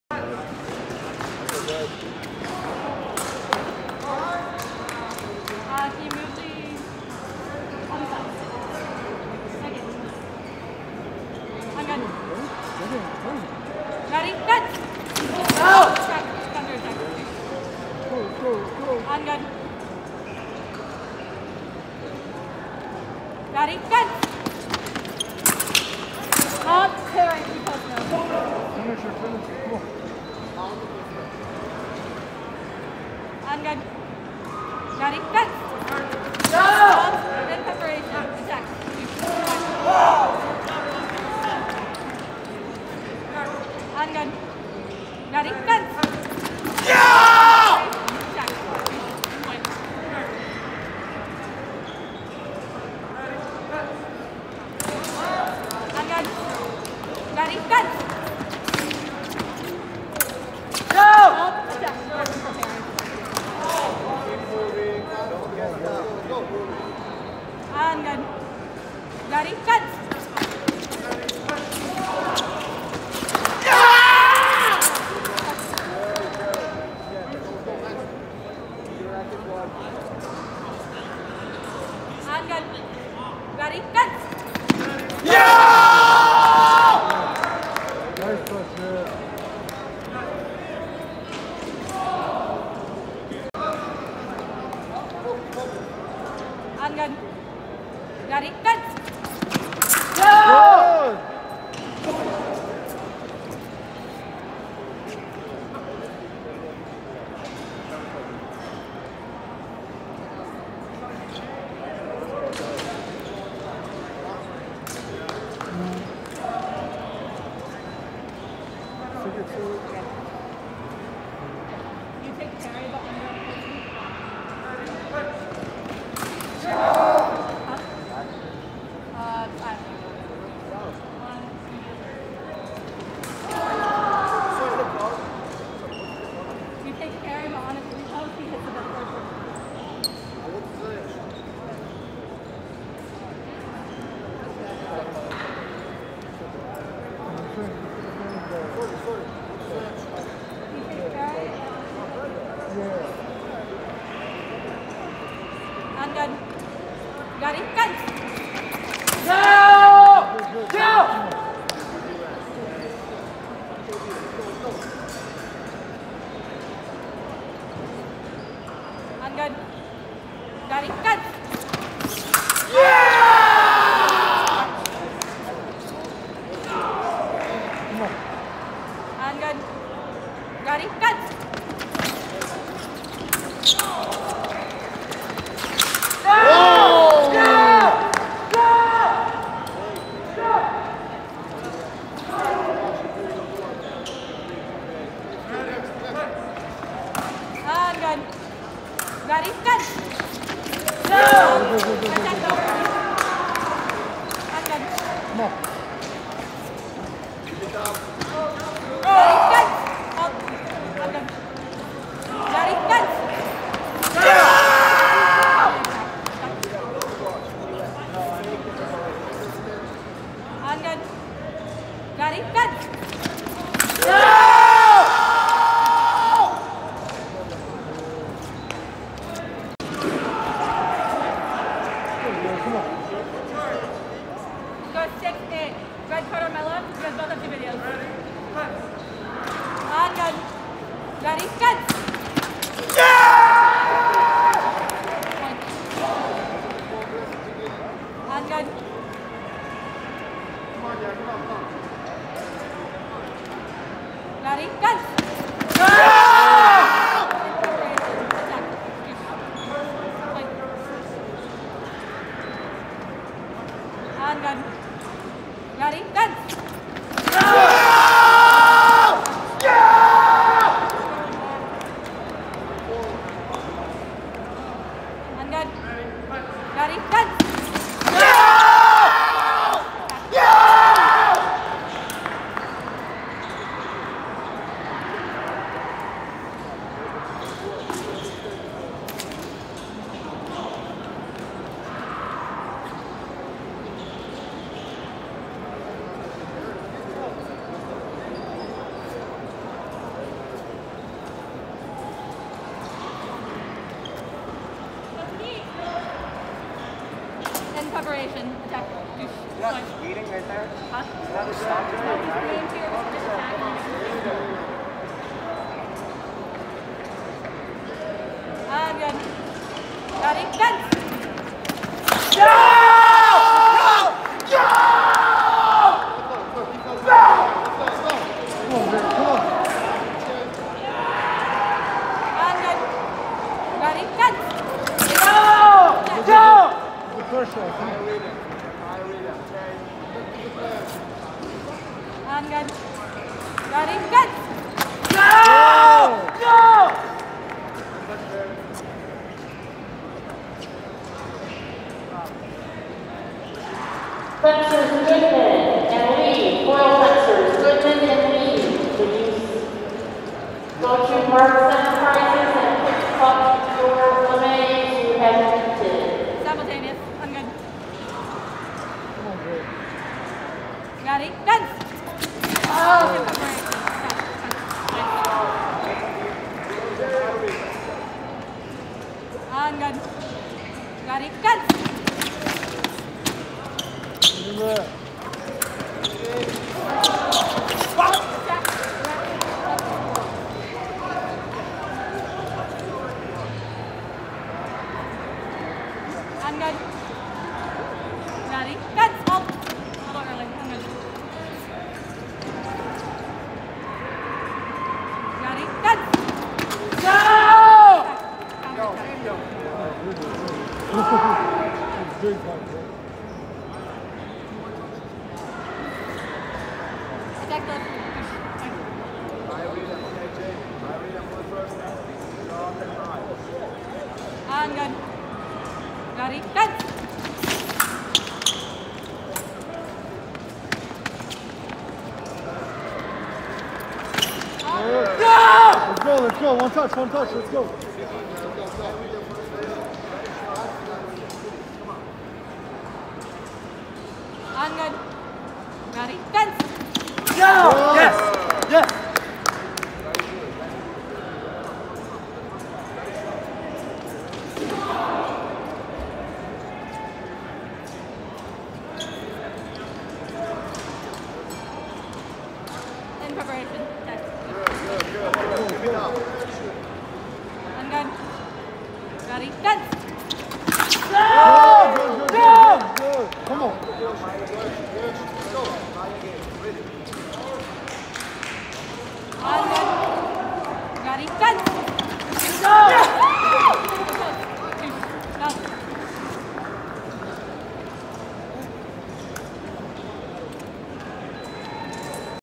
Columbus NAC SMS 2021 - L16 - Jared Smith v Kamar Skeete
This is a L16 bout in the senior men's sabre event at the December NAC in Columbus, Ohio. Jared Smith is on the left and Kamar Skeete is on the right.

Thank you to Eddie Maklin for filming this bout!